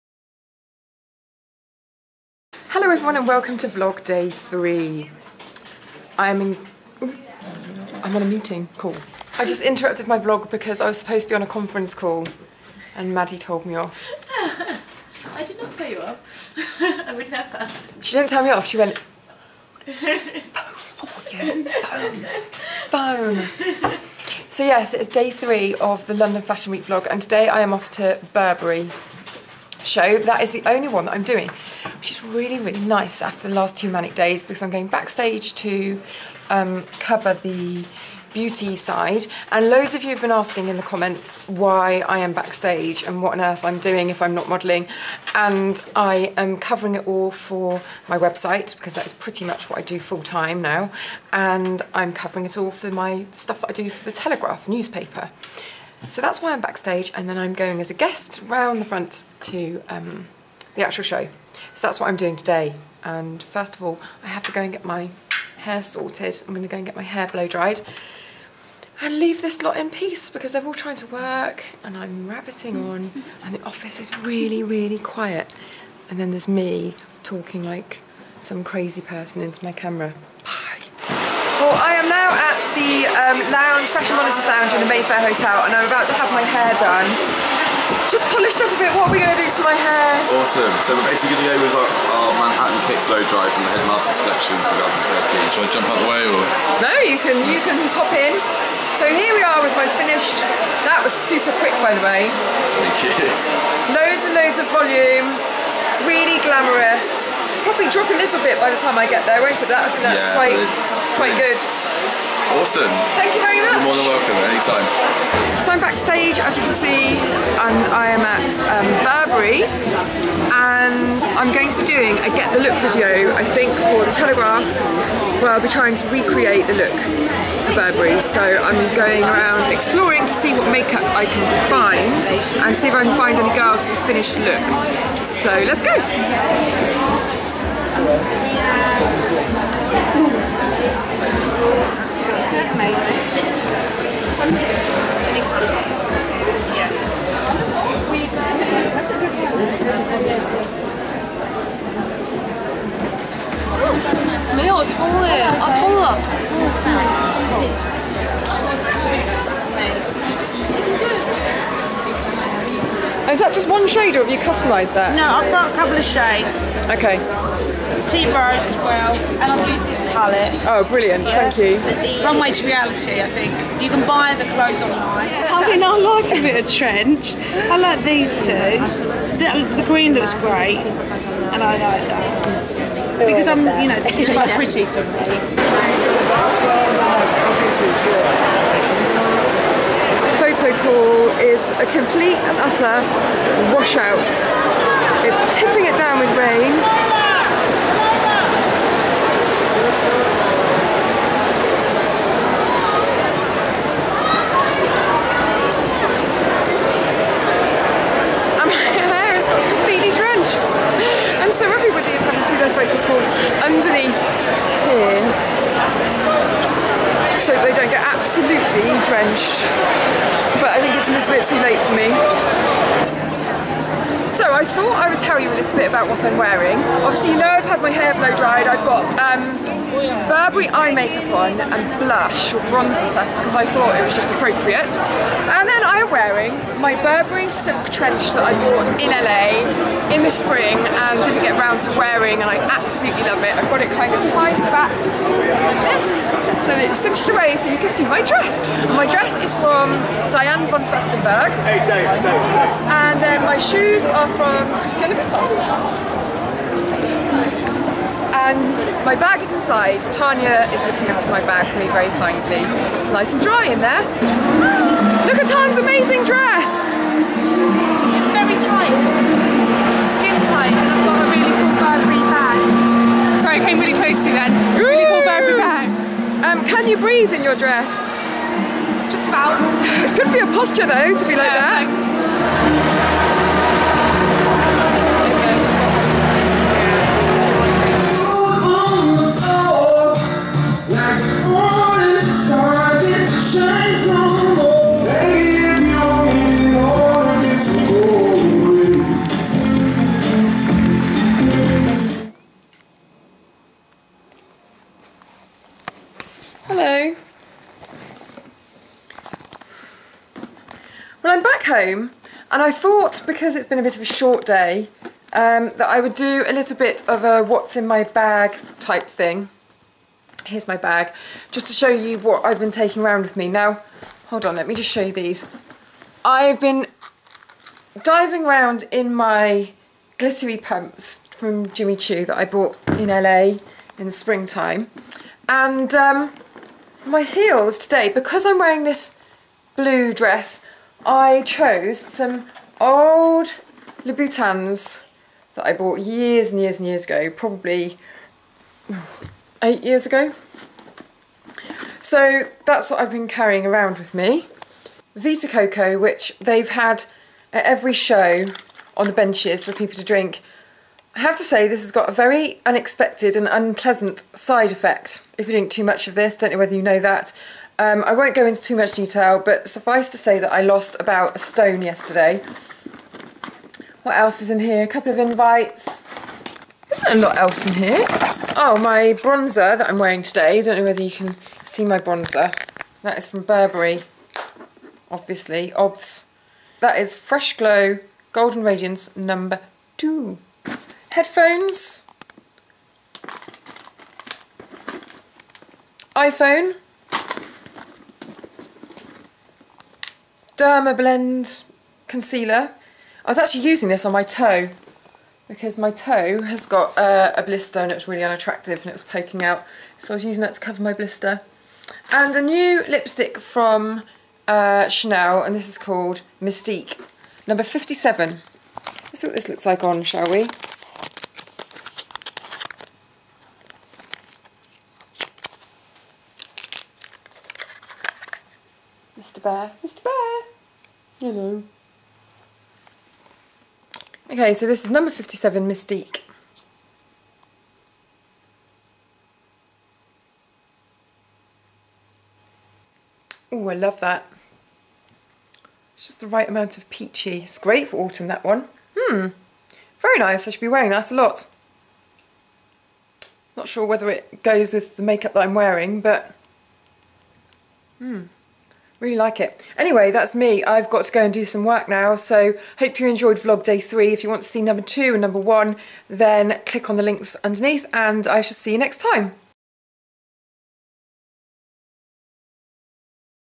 Burberry, Blowdries and Bear! Vlog Day 3
Coat is by Burberry
Dress is by Diane von Furstenberg
Shoes are Christian Louboutin
Bag is Michael Kors

Hair kindly styled by Headmasters at the Fashion Monitor Lounge

Thank you to Lucy for filming my outfit!